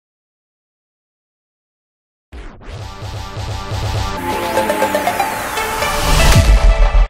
Quick Flat Glitch | VideoHive Templates | After Effects Project Files
Suitable for Corporate ids, Promos, Intro Videos etc…

Project features: Clean, cool and trendy design No plugins required Extremely fast rendering Very easy to use Color change You can easily add your logo or text Full HD resolution (1080p) CC 2014, CS6, CC compatibility Help file included